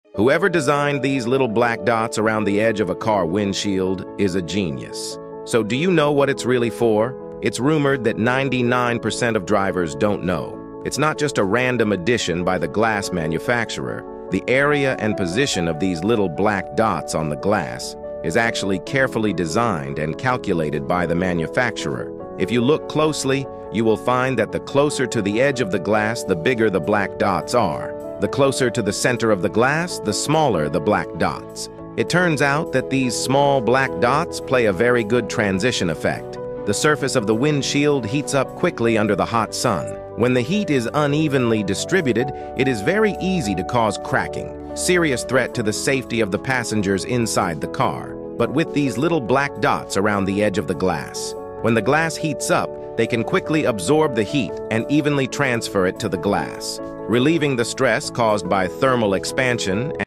The Reason Behind Dots on Car Windshields
Unveiling the Mystery: The Reason Behind Dots on Car Windshields"

Description:
Embark on a journey of discovery as we delve into the intriguing world of those tiny dots adorning car windshields. Ever wondered why they're there? This short video reveals the fascinating purpose behind these seemingly insignificant features. From enhancing safety to structural integrity, learn why these dots are more than just decoration. Join us as we uncover the hidden secrets behind every windshield's dotted design!